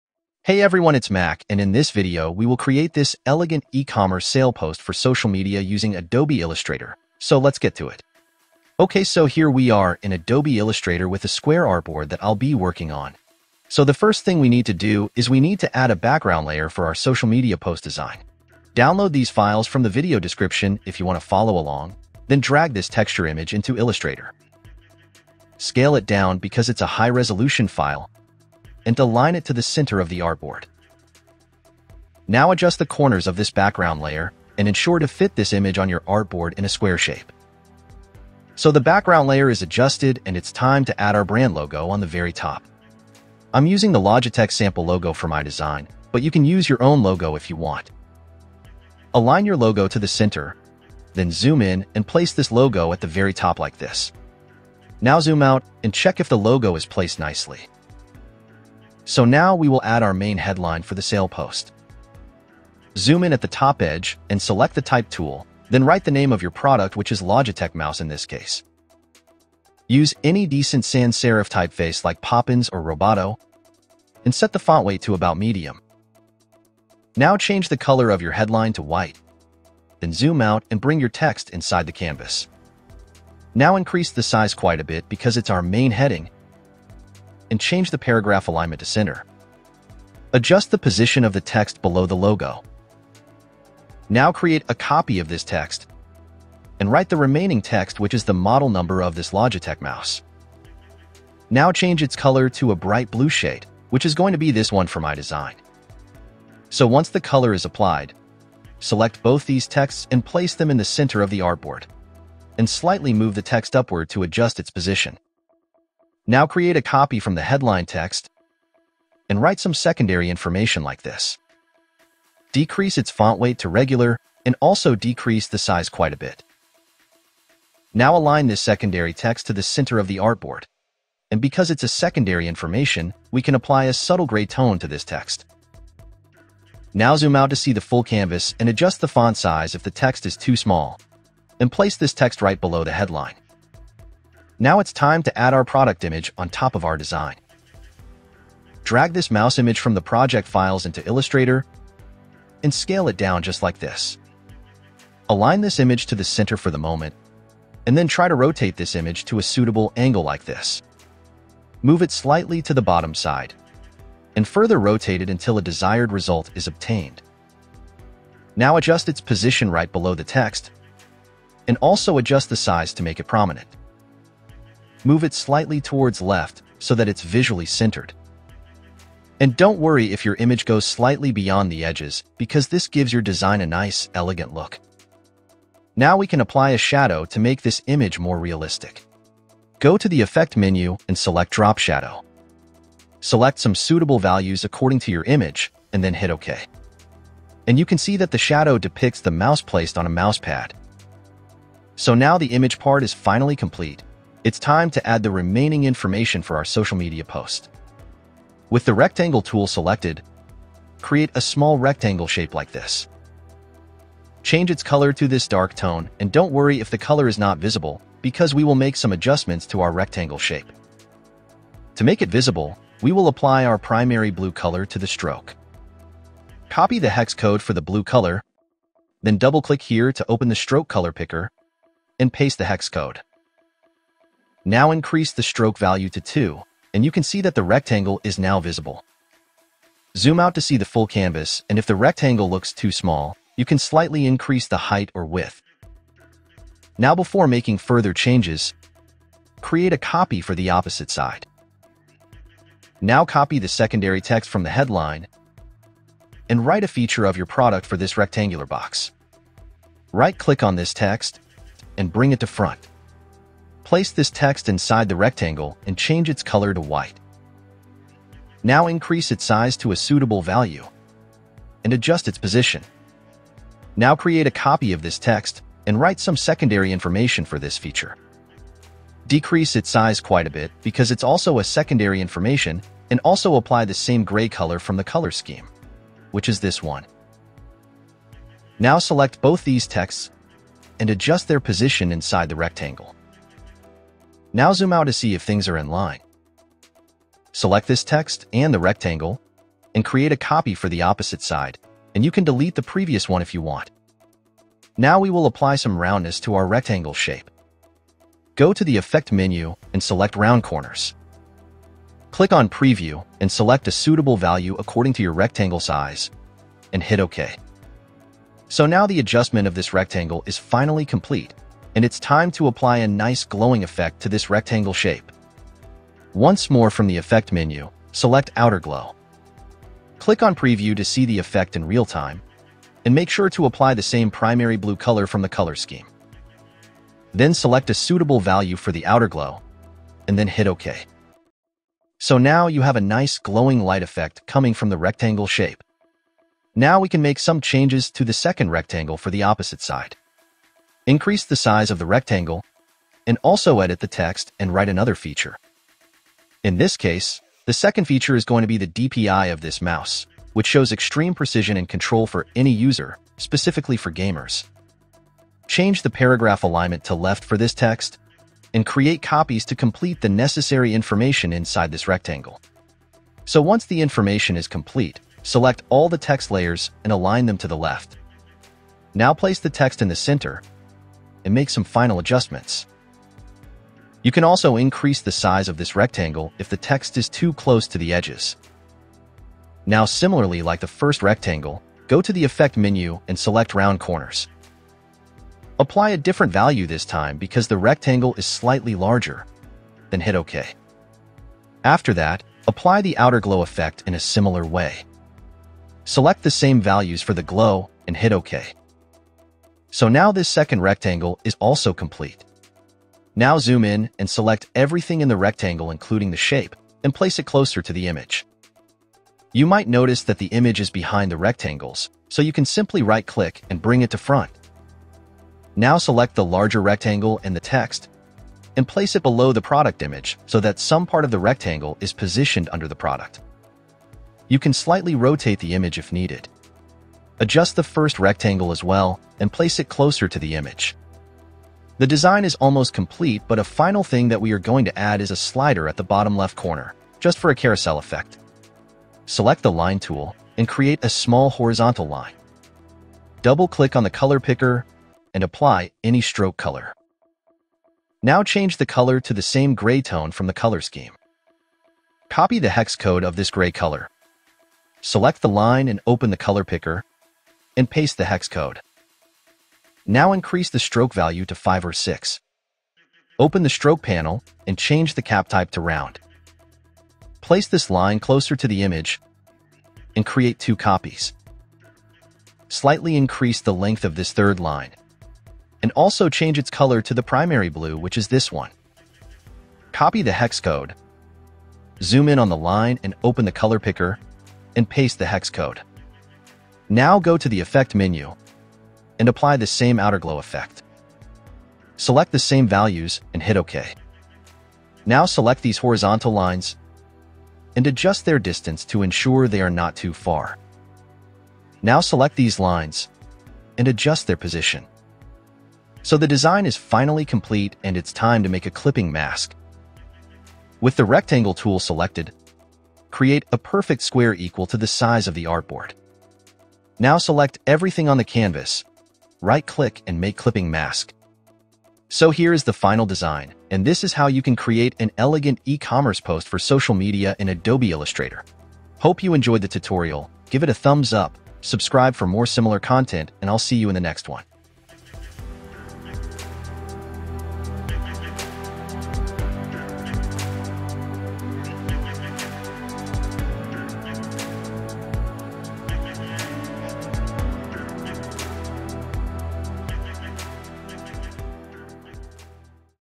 Social Media Post Design Tutorial For Beginners | Illustrator
Welcome to my Adobe Illustrator tutorial for beginners!

🎨 Learn How to Design Ecommerce Product Sale Social Media Posts & Banners in Adobe Illustrator!

In this tutorial, I'll guide you through creating eye-catching designs perfect for boosting sales online. Whether you're a beginner or seasoned designer, this step-by-step tutorial covers everything from layout to typography and essential design tips.

🔹 Discover the best practices for creating compelling social media posts that drive engagement.
🔹 Master Adobe Illustrator tools and techniques tailored for ecommerce promotions.
🔹 Design professional banners that captivate your audience and increase click-through rates.
🔹 Utilize special effects like the outer-glow effect to make your designs pop!

Key Topics Covered:
- Creating dynamic layouts that highlight product features
- Using Adobe Illustrator's tools effectively for ecommerce visuals
- Enhancing your designs with typography tips and tricks
- Applying the outer-glow effect for a polished and professional look

Don't miss out on maximizing your online presence with effective design strategies! Watch now and elevate your ecommerce game today.